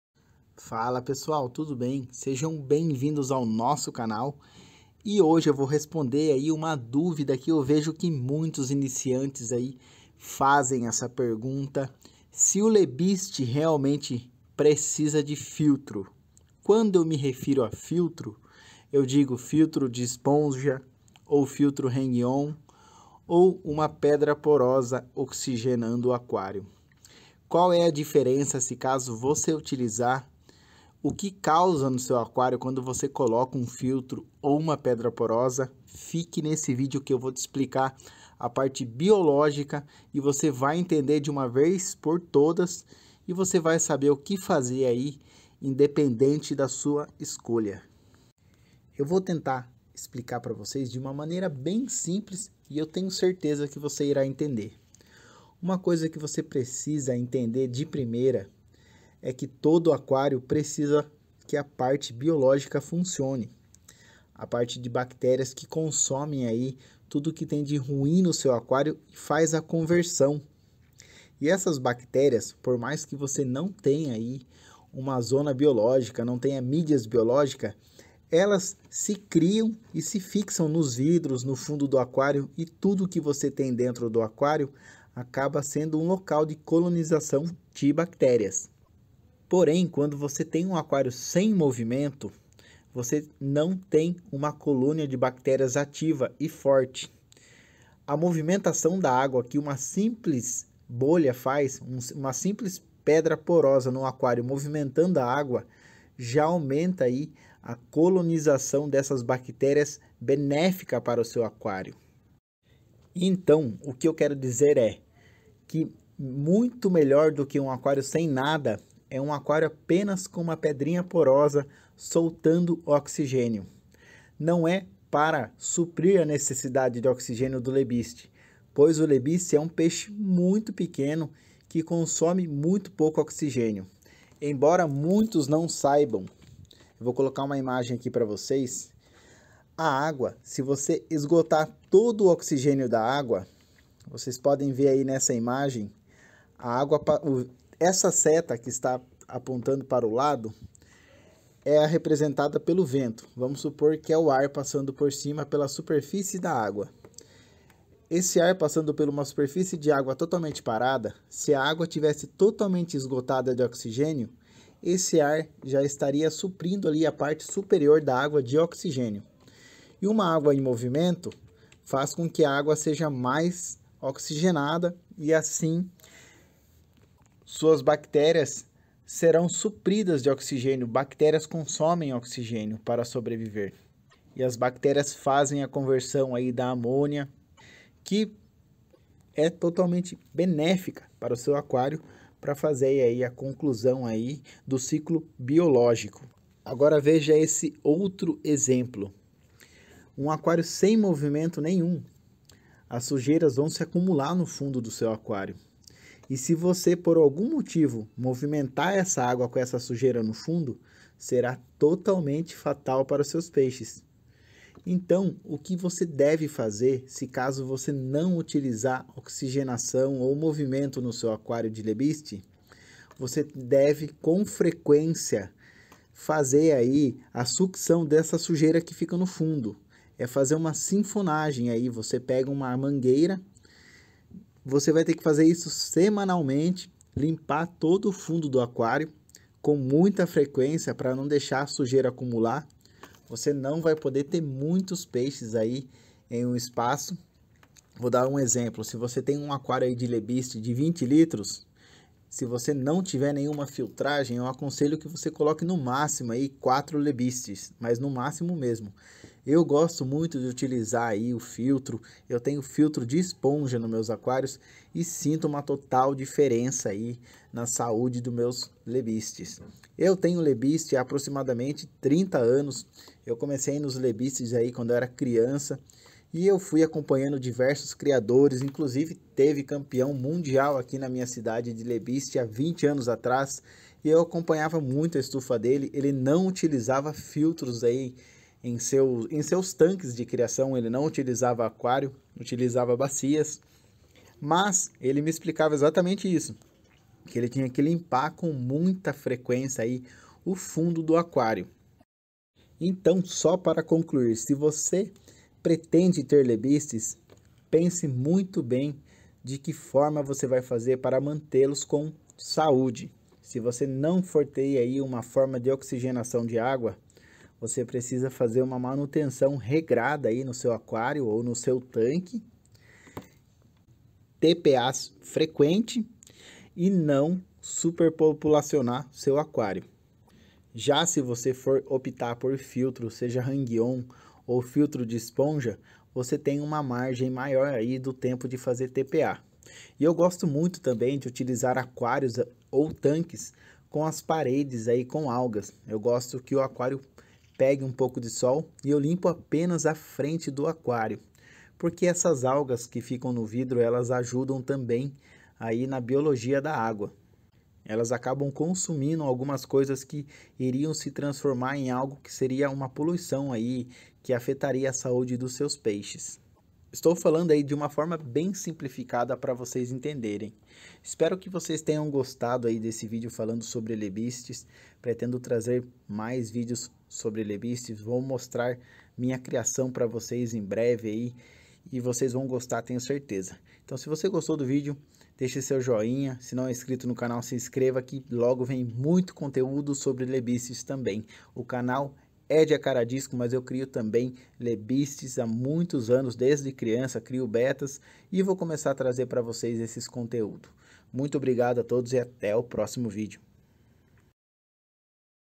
GUPPY PRECISA DE FILTRO? ENTENDA
Nesse video Irei explicar um pouco  as duas opções.
Guppy com filtro  sem filtro,  O que isso vai ipactar em seu aquário e na biologia, e na saude dos seus peixes.